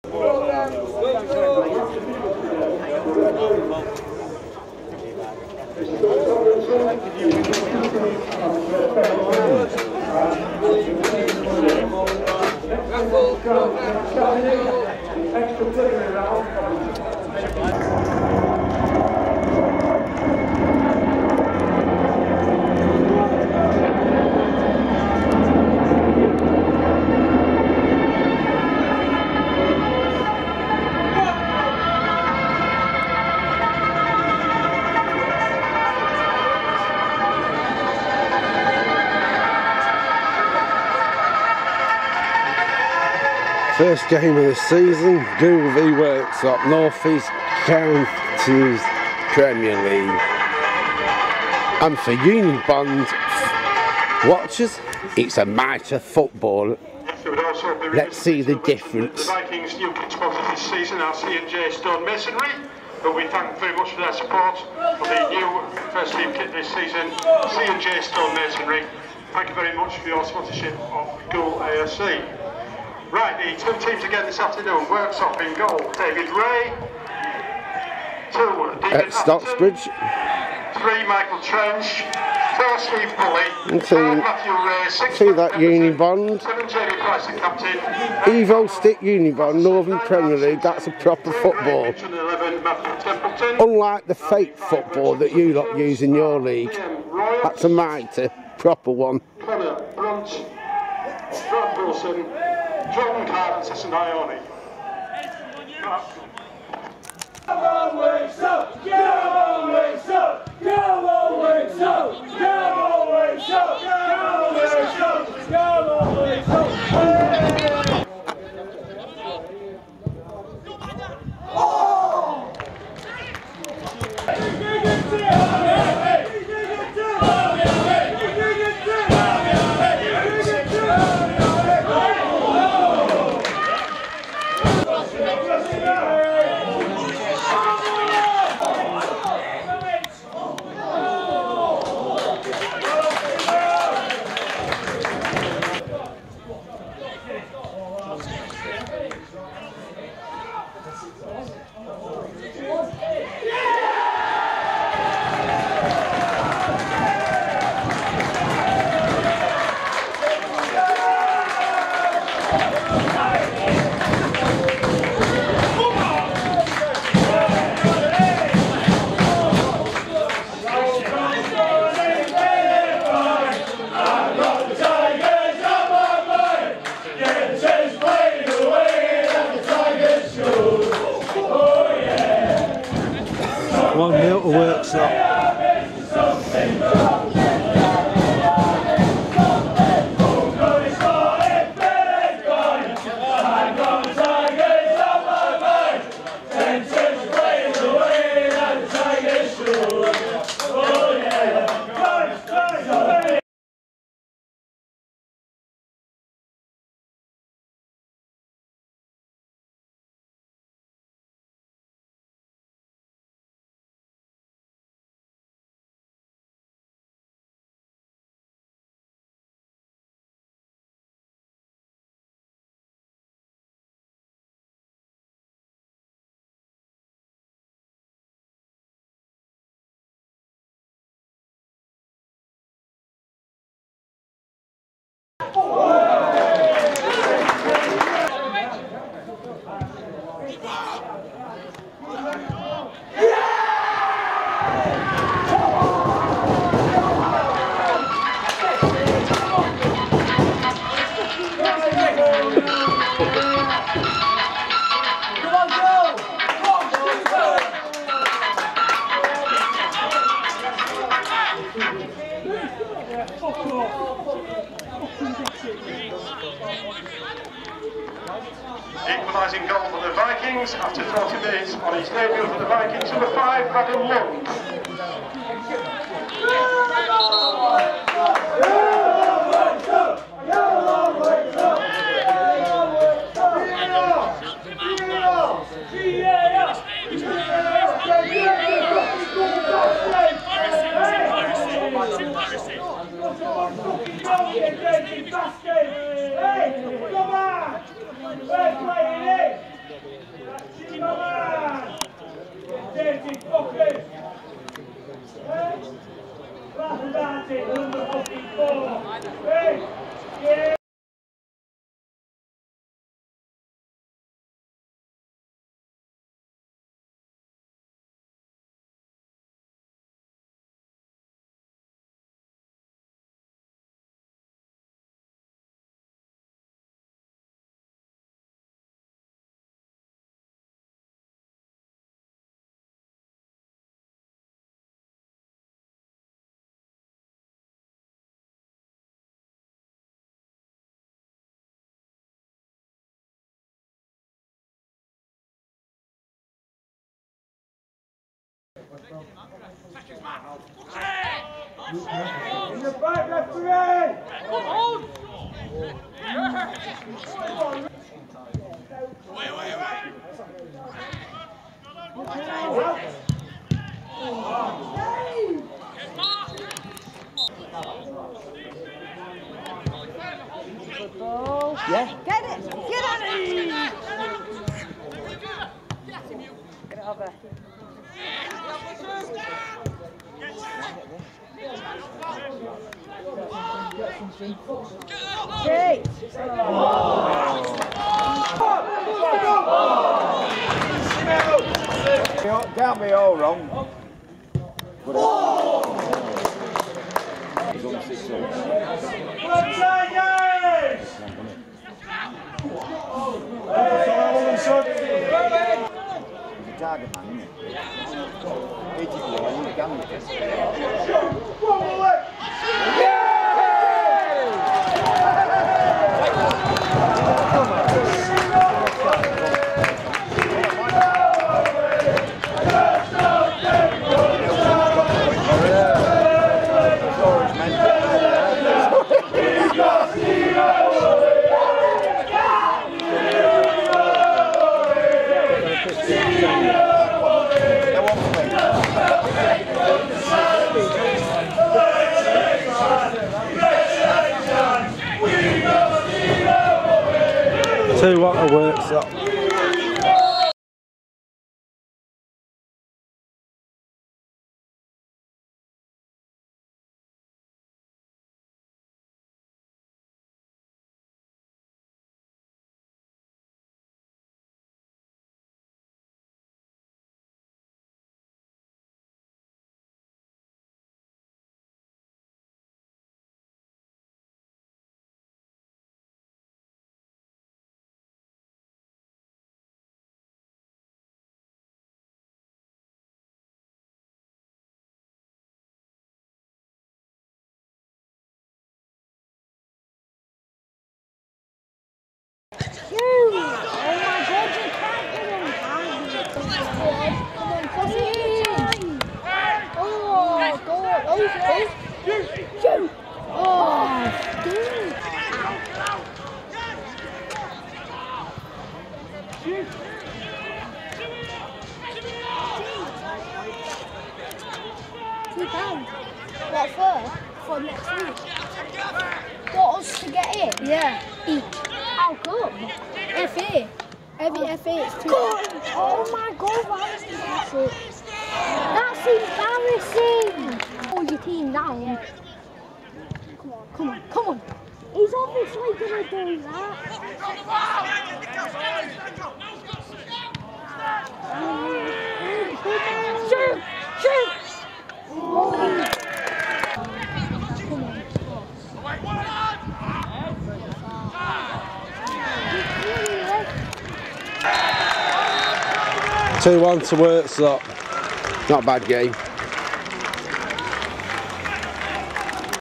Goole AFC 1 v 2 Worksop Town FC 04 08 2018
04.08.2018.
Northern Counties East League Premier Division .
Goole AFC 1 v 2 Worksop Town FC .
Victoria Pleasure Grounds .
ATT 336